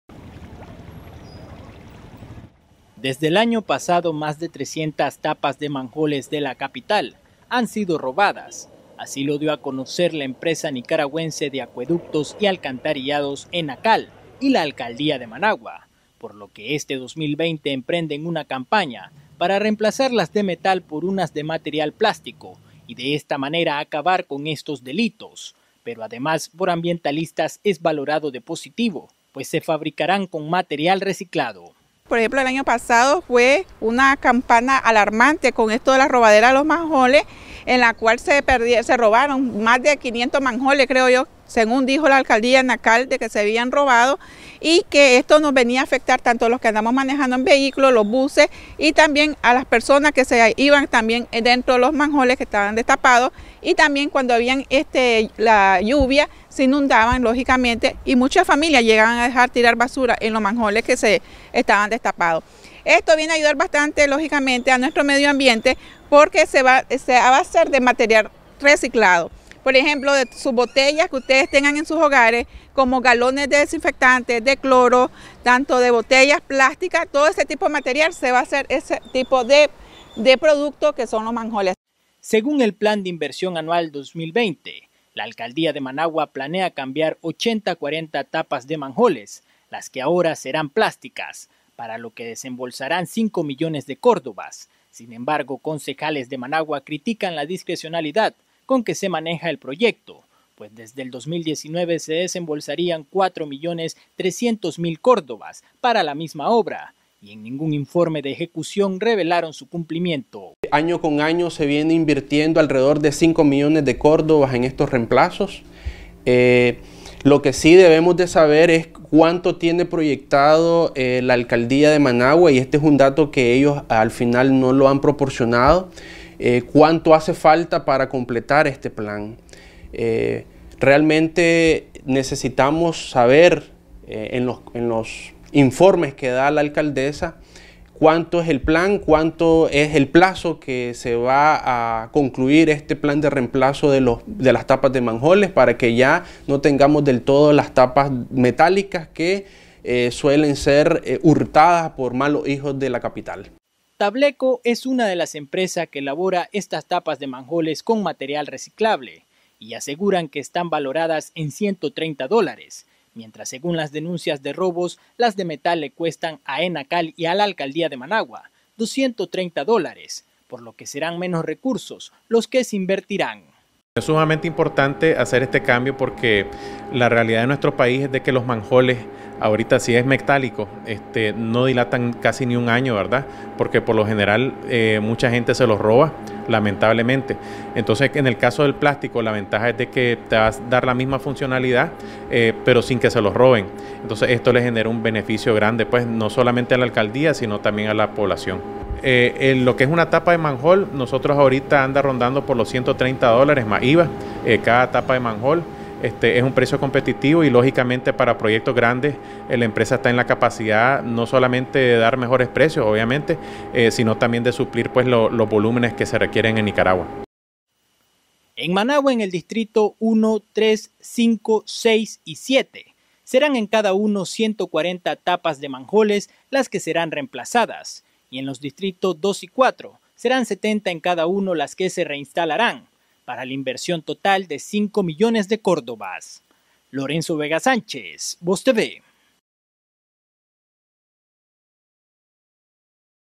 Tapas metálicas de alcantarillas serán reemplazadas por plásticas
La alcaldía de Managua reemplazará al menos 840 tapas metálicas de las alcantarillas por plásticas, debido a que el año pasado más de 300 fueron robadas.